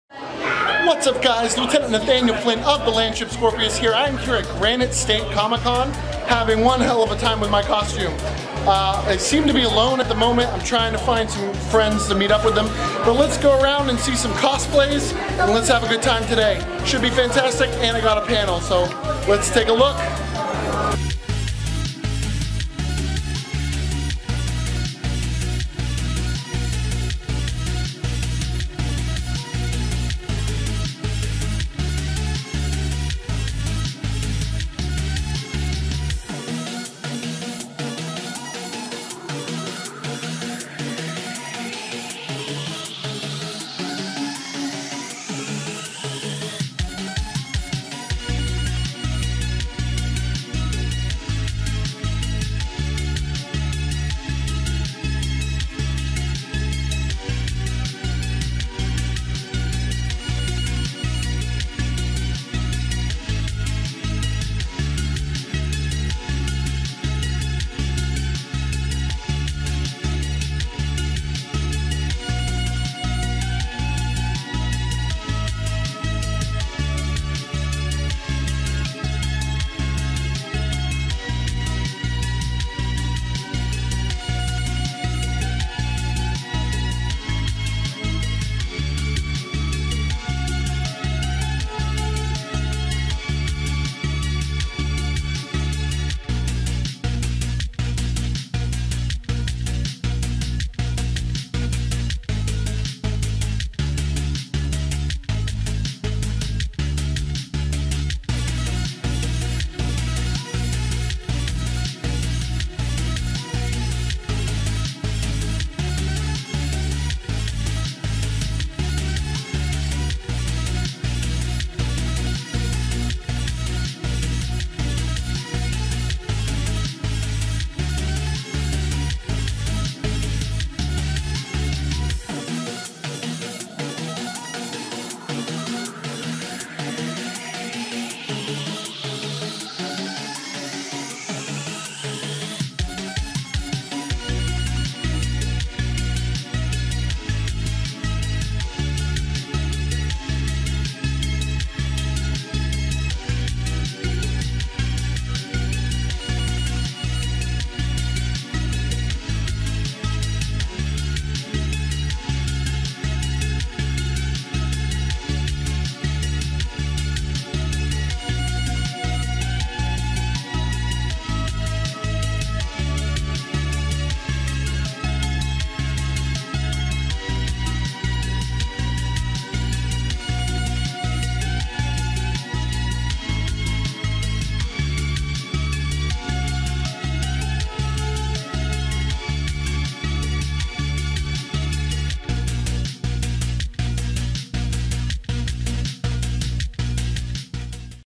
Granite State Comic Con 2018
The Landship Scorpios tried to have its cake and eat it too at the Granite State Comic Con. However, we got the short end of both sticks and ended up failing.

Enjoy what footage I was able to get about this fantastic event. Cosplays, vendors, and panels galore!